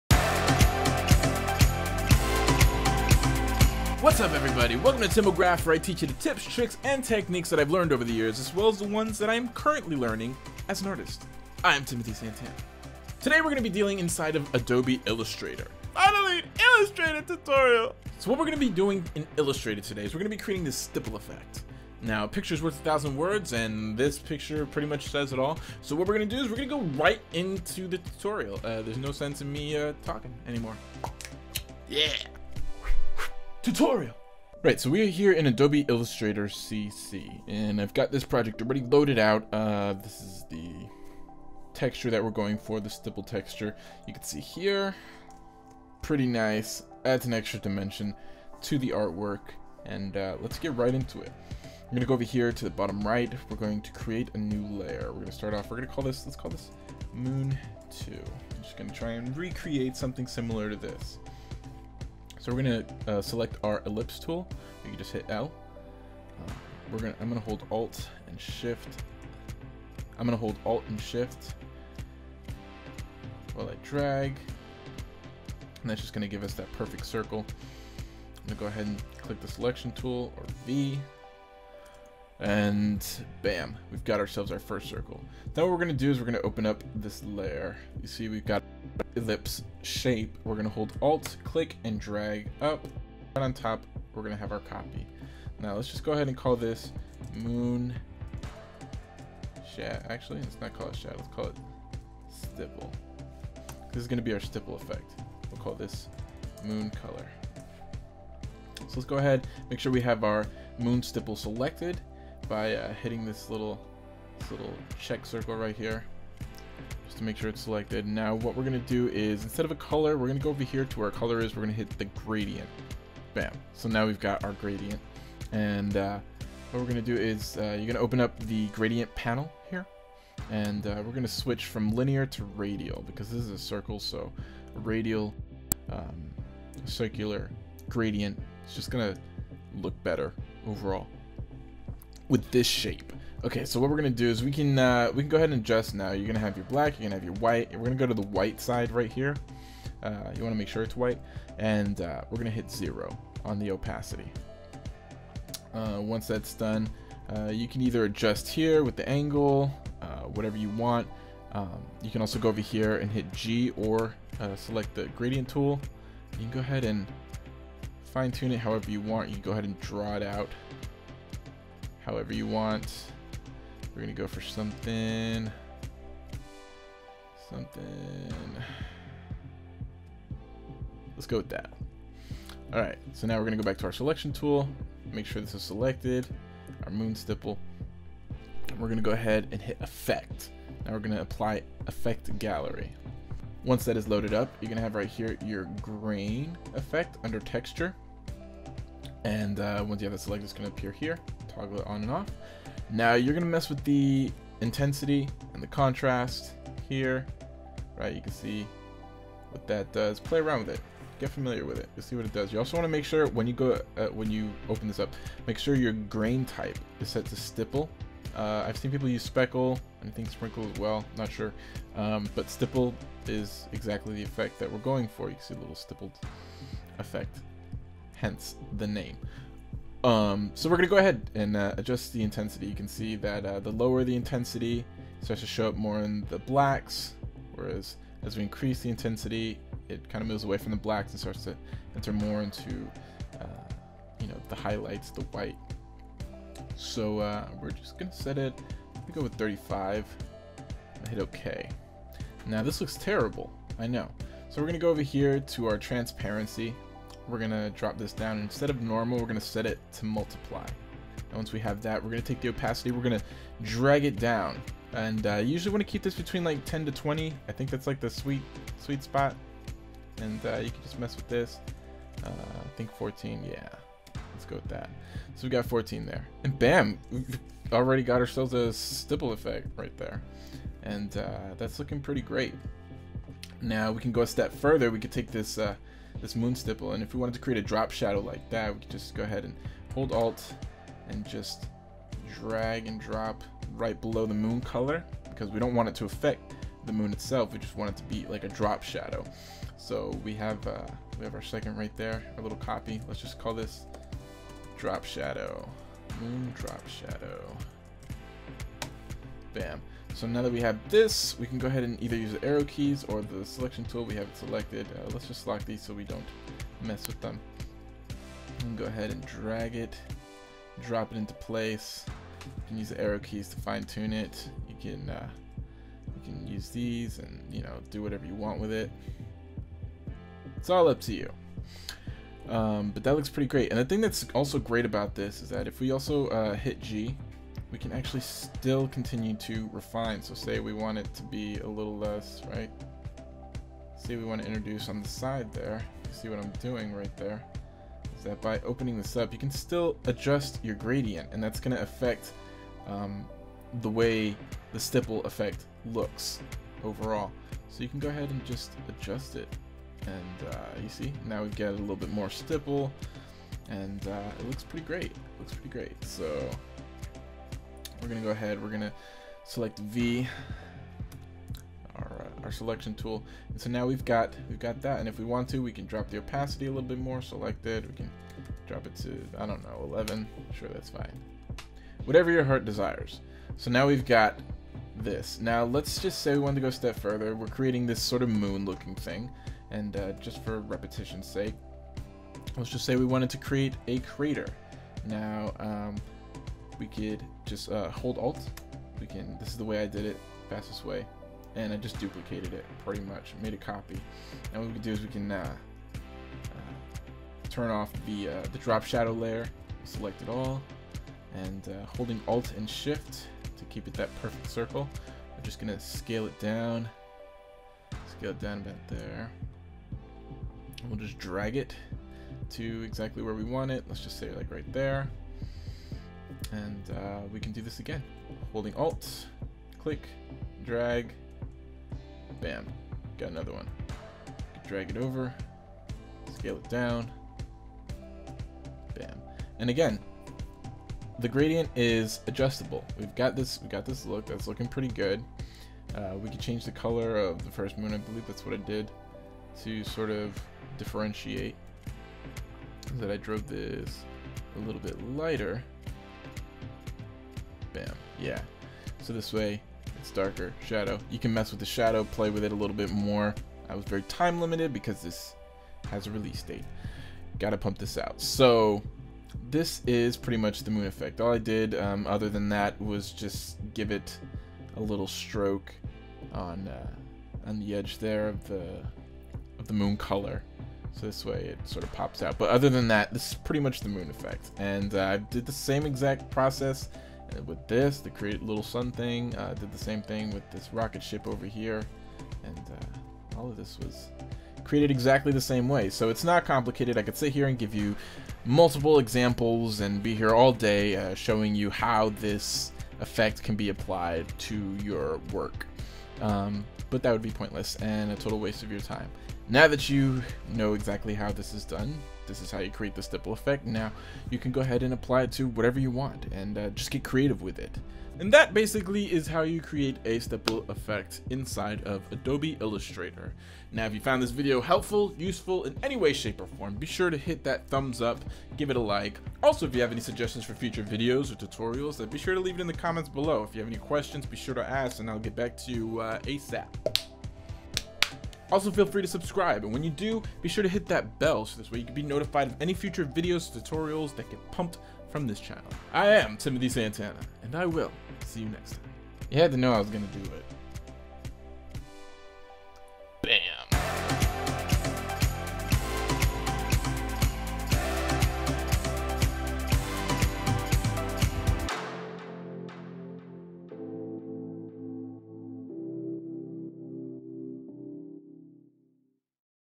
How to: Stippled Texture Illustration Effect | Adobe Illustrator Tutorial
Looking to add a little more oomph to your illustrations/graphic art? Looking for a retro look? Looking for FREE money!? Looking for how to create a dotted/stippled texture for your illustrations without having to draw each individual dot!!!?

Well, good news! You clicked on the right video! That's if you're looking to create a stippled effect cause I haven't found the free money thing yet... But if anyone else does, be sure to let me know! ;)

Hope you enjoyed the tutorial and found it useful!